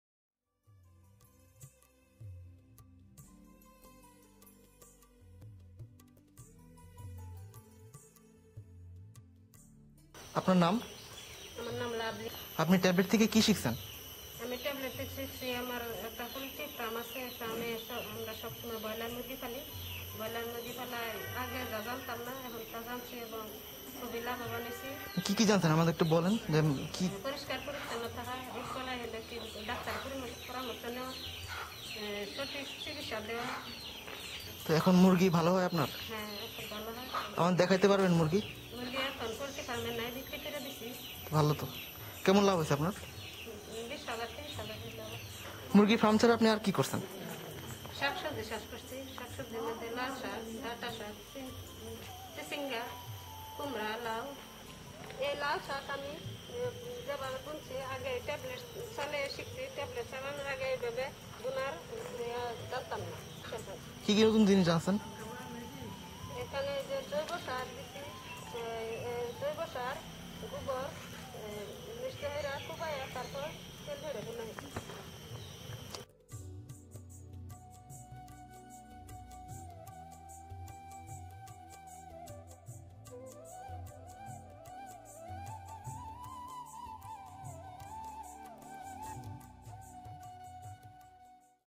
Poultry Farming and Kitchen Gardening - Women in SME
Mrs. Lovely is a poultry farmer who has benefited from the information she received from the tablet on raising chickens. In addition she learned how to grow her own red spinach which she and her family will be able to enjoy soon.

Lovely and her family are participating in a four-month long "Digits to All" project funded by Amadeyr Cloud Limited. The focus of the project is to disseminate information using technology. Using a simple touch screen tablet, households can learn about: health issues; education; agriculture (also poultry/ fish/cattle farming); and general information such as government services/women's rights/ and small enterprise development. The majority of the information on the tablet is in audio/visual form so that semi literate and illiterate persons can easily understand the content.
The tablets have been lent to the families in Lovely's area (like a library book), for the entire family to use, and then be returned at the end of the project.